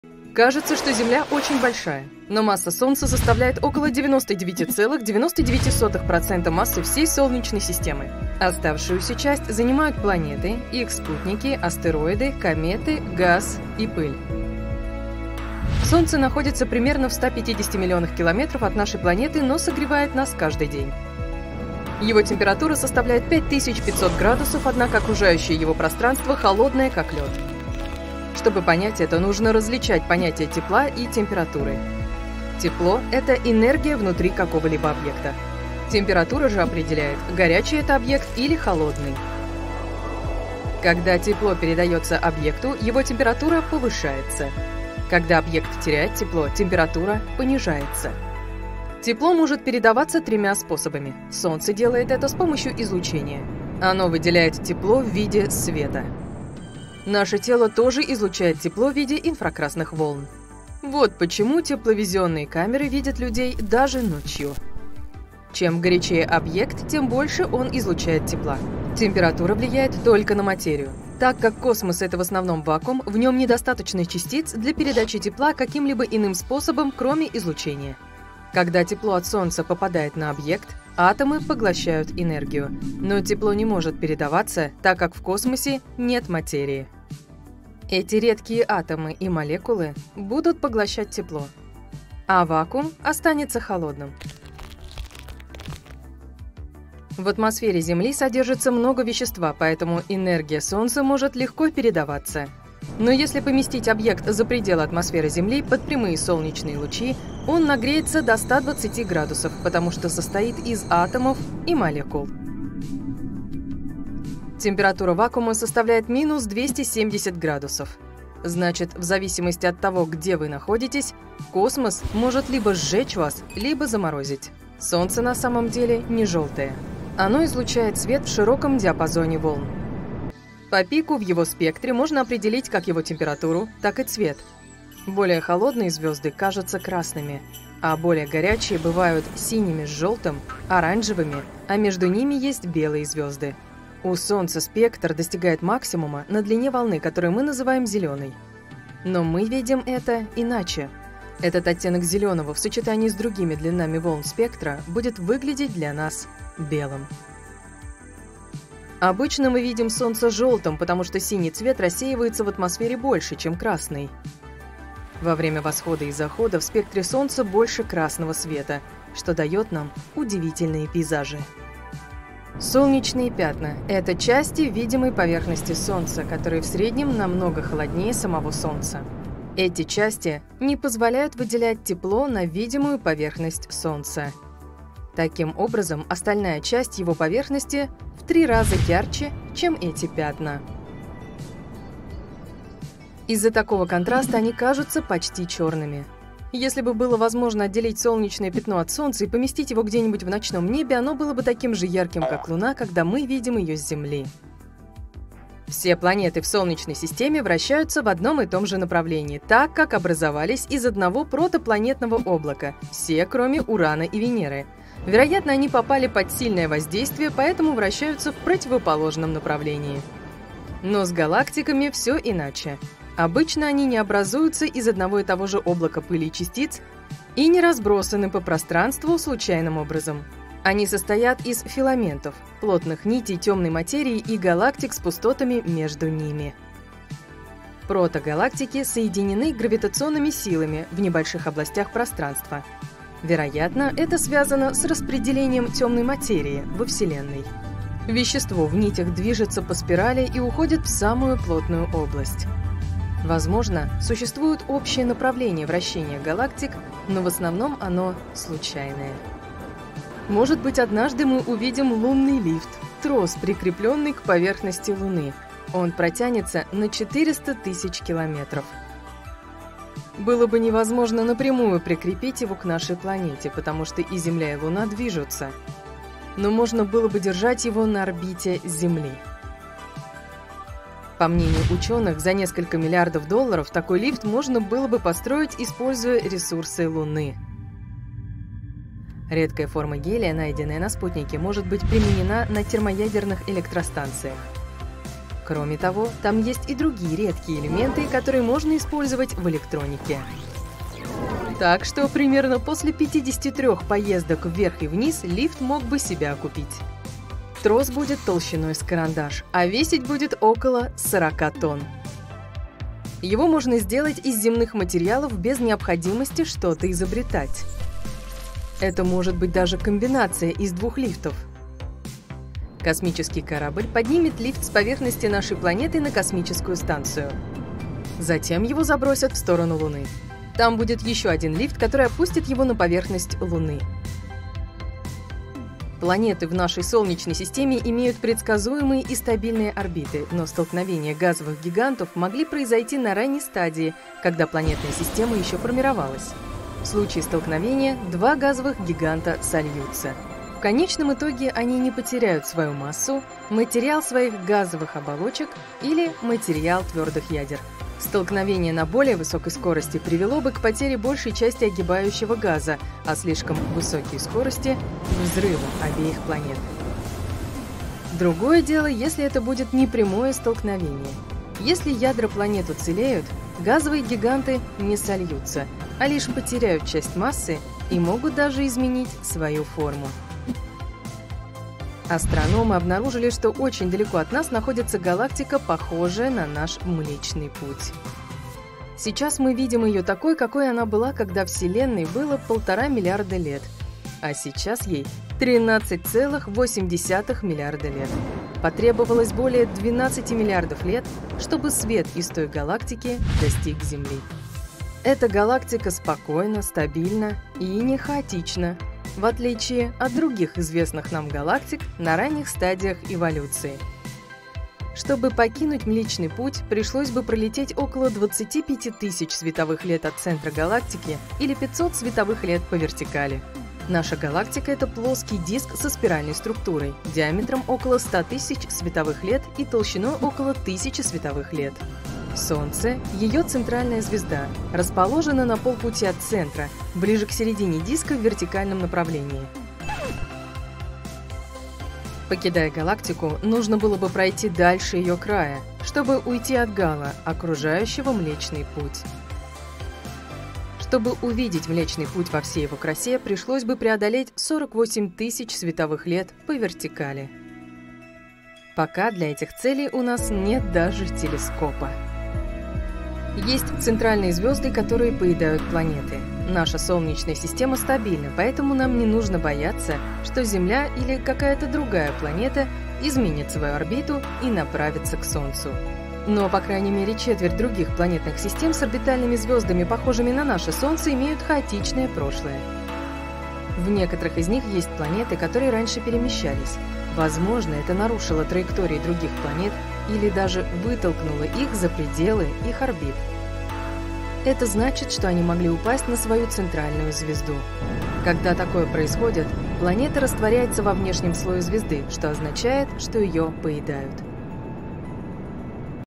Почему в космосе так холодно, если Солнце такое горячее?🤔
Почему в космосе холодно несмотря на жаркое Солнце? Ведь большую часть года оно прекрасно согревает нас на Земле – так почему же оно не может согреть пространство? Причина в том, что космос представляет собой идеальный термос. Давайте выясним, что это значит и каким образом пространство остается холодным, как лед!